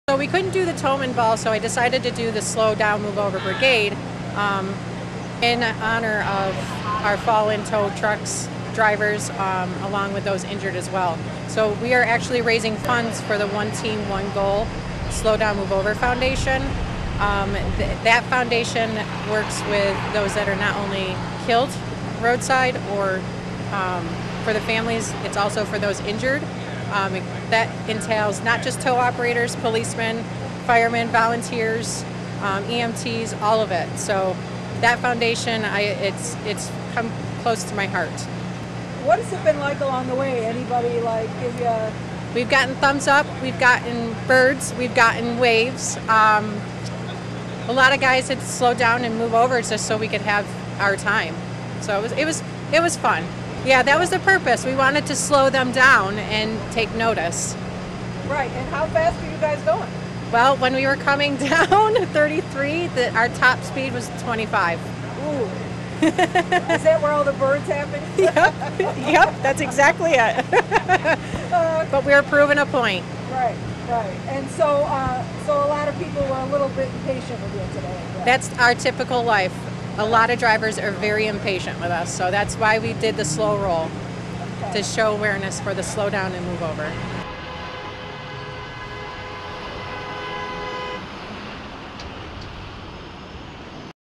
BATAVIA/Slow Down Move Over Brigade
Callahan Towing organized a Slow Down Move Over Brigade on Saturday morning. Area tow operators traveled from Rochester, beginning at the Market Place mall, to Buffalo, with a stop in Batavia at noon.

The group of tow trucks traveled about 25 mph the entire trip, a slow role, to make drivers pay more attention to tow operators on the road.

"We wanted to slow them down and take notice," says organizer Sarah Callahan of Callahan towing.

Callahan says that on Rt. 33 from Bergen to Batavia, the group received a lot of arm gestures, "birds" , waves and honks, which Callahan says is a typical day for tow operators.

The slow roll event today also raised funds for One Team One Goal/Thin Line SDMO Foundation Inc.